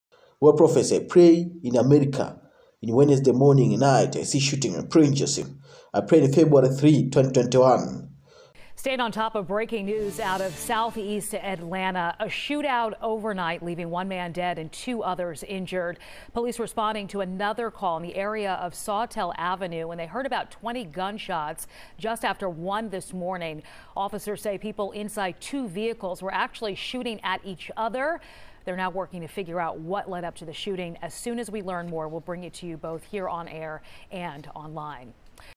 February 4, 2021 Man In Killed in Atlanta in Triple Shooting Prophecy
January 31, 2021 Sunday Of World
Prophecy
January 31,2021
World Prophecy.🇺🇸
I Praying in America Wednesday Morning and Night i see Shooting and I Praying in Jesus Name in February 3,2021.

February 4,2021
Man Killed  Atlanta in Triple Shooting inside two vehicles Wednesday Morning
in February 3,2021.

GoodMorning.TV

good.morningtv
GoodMorning.TV

Good Morning. TV